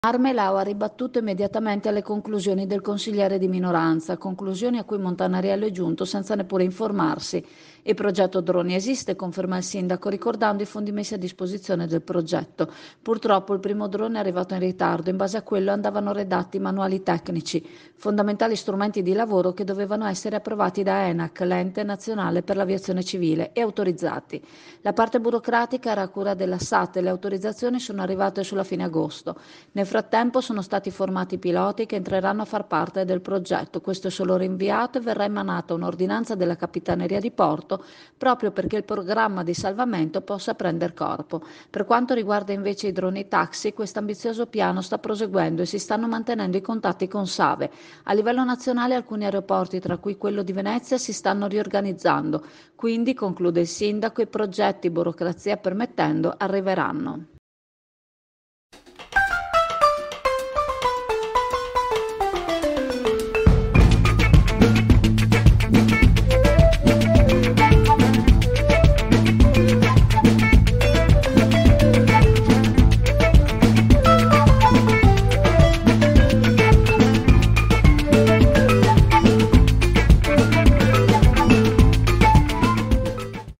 ARMELAO DRONI PRIMA DI PARLARE MONTANARIELLO SI DOVREBBE INFORMARE
Ma Armelao ha ribattuto immediatamente alle conclusioni del consigliere di minoranza, conclusioni a cui Montanariello è giunto senza neppure informarsi.
Il progetto droni esiste, conferma il sindaco, ricordando i fondi messi a disposizione del progetto. Purtroppo il primo drone è arrivato in ritardo e in base a quello andavano redatti i manuali tecnici, fondamentali strumenti di lavoro che dovevano essere approvati da ENAC, Ente Nazionale per l’Aviazione Civile, e autorizzati.
La parte burocratica era a cura della SAT e le autorizzazioni sono arrivate solo a fine agosto. Nel frattempo sono stati formati i piloti che entreranno a far parte del progetto. Questo è solo rinviato e verrà emanata un’ordinanza della Capitaneria di Porto proprio perché il programma di salvamento possa prendere corpo.
Per quanto riguarda invece i Droni Taxi , quest’ambizioso piano sta proseguendo e si stanno mantenendo i contatti con SAVE.
A livello nazionale alcuni aeroporti, tra cui quello di Venezia, si stanno riorganizzando, quindi, conclude il sindaco, i progetti, burocrazia permettendo, arriveranno.